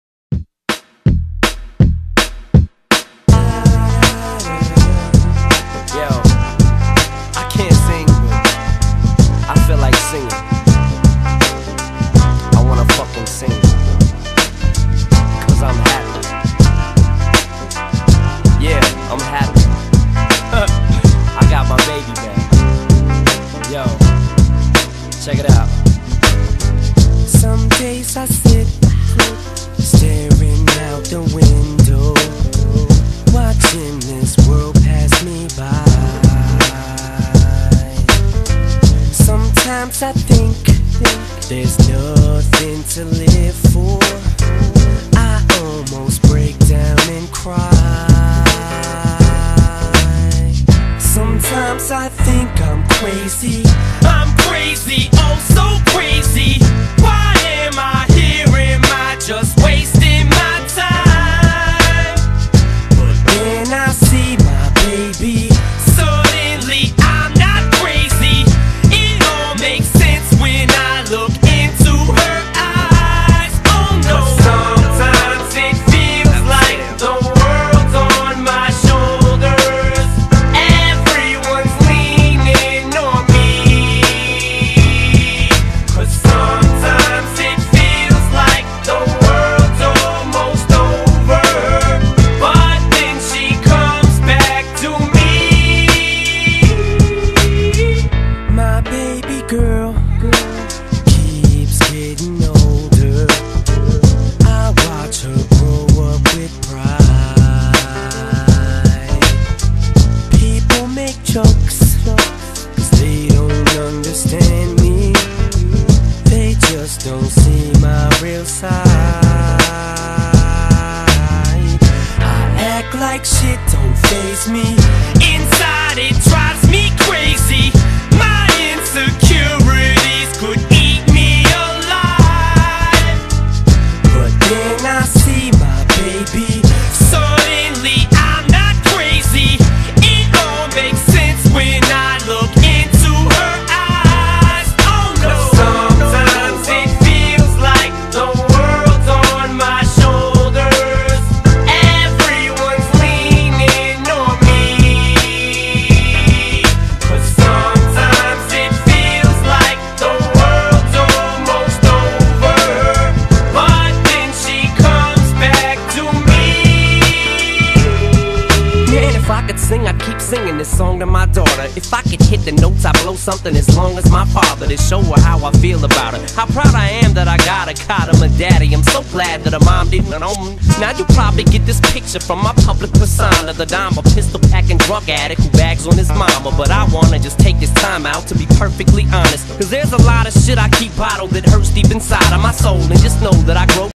Sometimes It Feels Like The Worlds On My Shoulders ;; Pikita.
I was meant to edit the bit where she slides out. LOL 8-)

Anyway, I know there's a lot of things wrong with my riding in the video. Probably a few wrong legs and diags and what not. Be a betch and crit if you must. Haha.

LAAVE YOU ALL! :)

xxxx